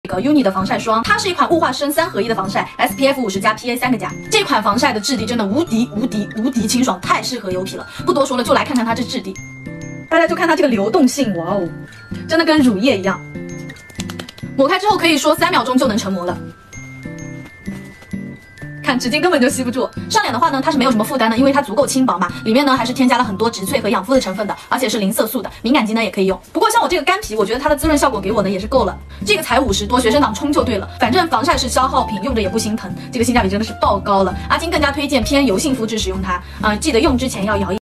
UNNY CLUB Sunscreen Lotion SPF50 PA+++ Review
Product description:
✅ The cream help protect against harmful UVA/UVB rays. Hydrated isolation, water resistant and sweat proof
✅ Contains plant extracts, which soothes, nourishes and moisturizes the skin effectively. It is a mild and low irritant sun cream, improving skin condition and keep the skin healthy.
✅ Tone up the skin and removes wrinkles of skin. Enhance your skin condition and keep it health.